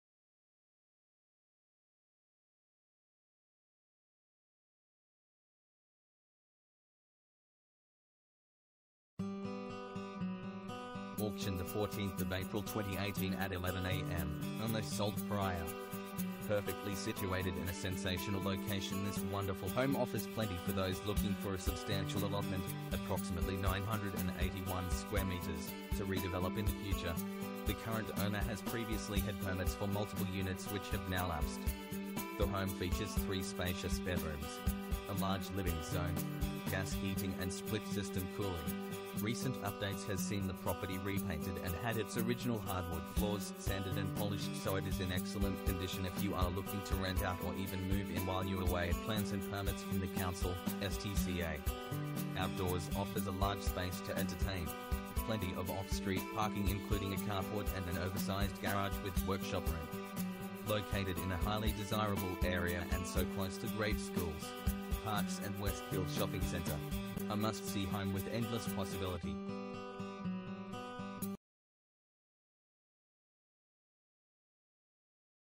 99 Kathryn Road Knoxfield, Victoria
Endless possibility on 981m2 (approx.). - AUCTION - 14/4/18 at 11am (unless sold prior)

Perfectly situated in a sensational location this wonderful home offers plenty for those looking for a substantial allotment (approx. 981m2) to redevelop in the future. The current owner has previously had permits for multiple units which have now lapsed.
The home features three spacious bedrooms, a large living zone, gas heating and split system cooling. Recent updates has seen the property repainted and had its original hardwood floors sanded and polished so it is in excellent condition if you are looking to rent out or even move in while you await plans and permits from the council (STCA).
Outdoors offers a large space to entertain, plenty of off street parking including a carport and an oversized garage with workshop room.
Located in a highly desirable area and so close to great schools, parks and Westfield Shopping Centre. A must see home with endless possibility!